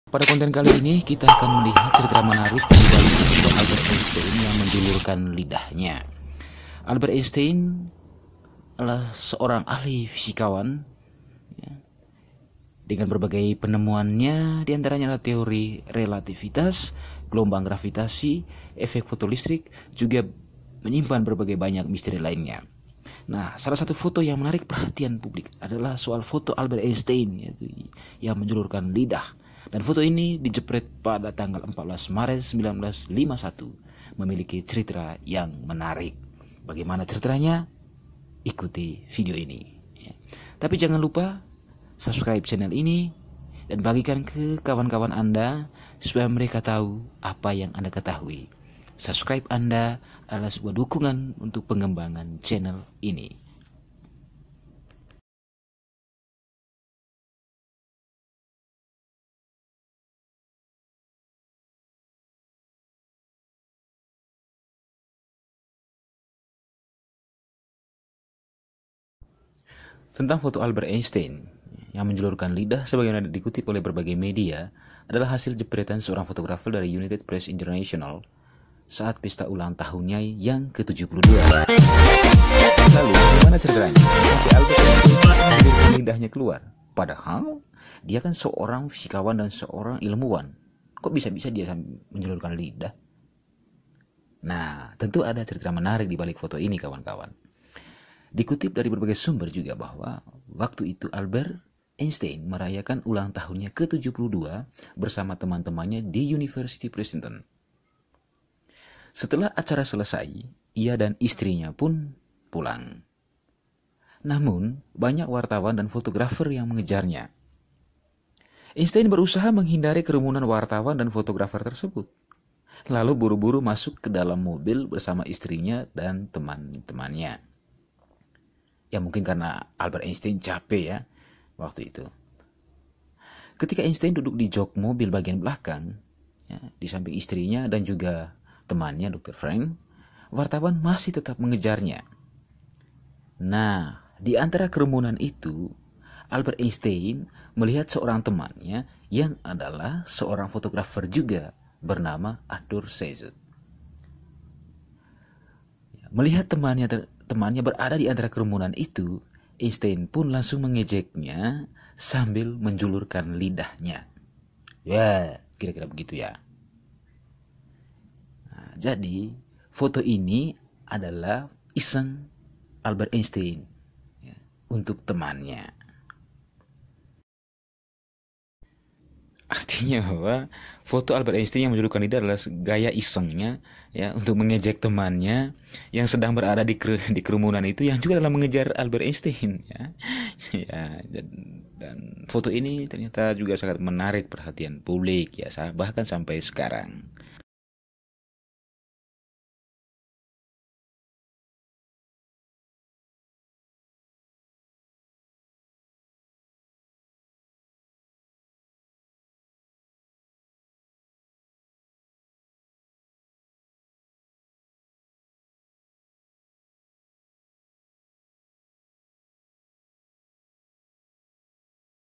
CERITA MENGEJUTKAN DIBALIK FOTO ALBERT EINSTEN MENJULURKAN LIDAHNYA || atta hattam TV
Terkadang ada foto yang menarik dan penuh arti,, yakni  kisah di balik foto tersebut. Seperti foto dari Albert Einstein yang menjulurkan lidahnya, yang kerap kali foto ini dijadikan meme dengan kata-kata yang berbeda. Gak cuma itu aja, masih ada lagi foto-foto ikonik